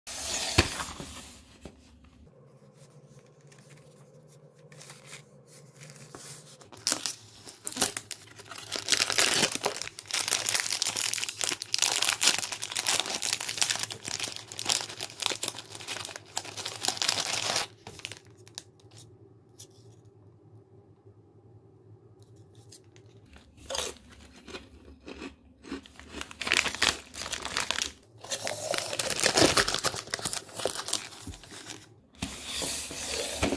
Japanese Bourbon Potelka Consommé Soup ASMR - 30 Day Chip Challenge (10)
Maybe soup isn't a good flavor to chips. It might be too subtle or I just have weak taste buds.